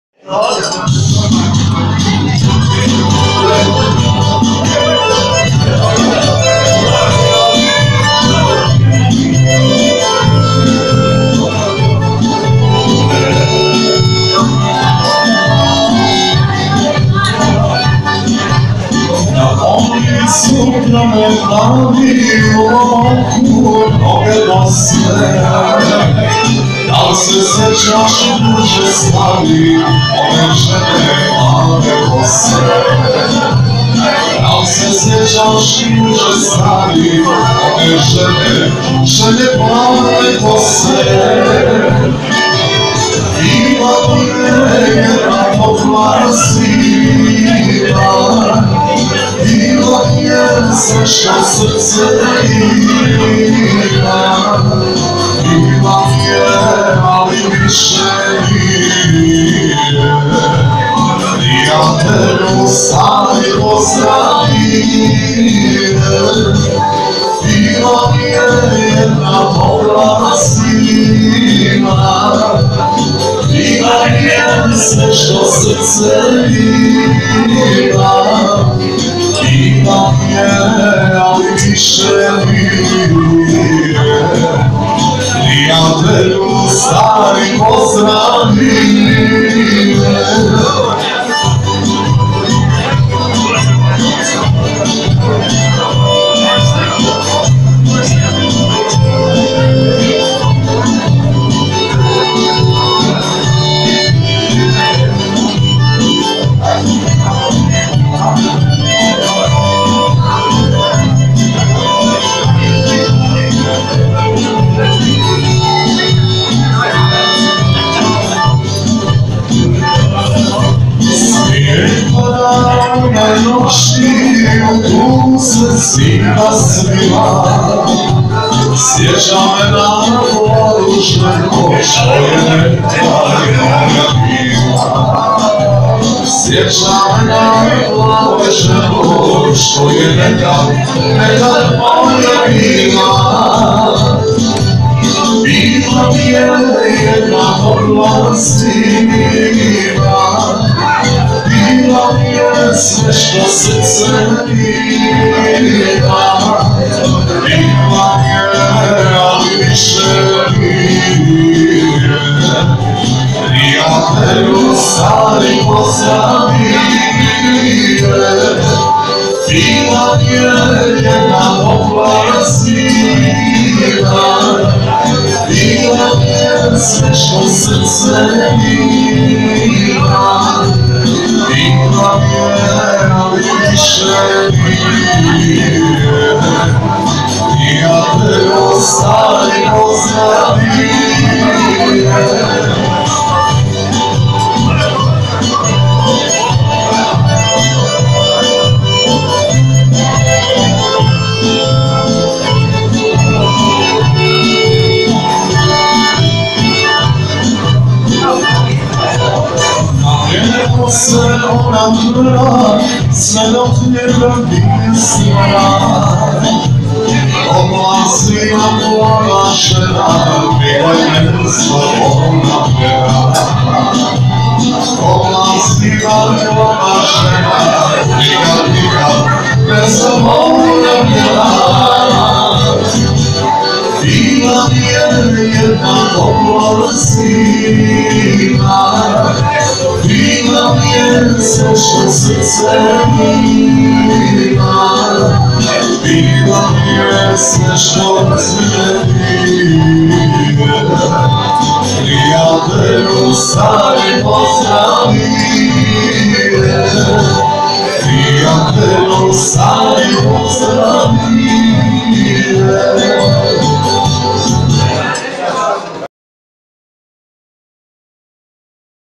Topla zima, 3.10.2015, Šljemenska
Šljemenska kod Tunje U Ljubačama. Izvode ;Pero, Alan-kuko i Sapko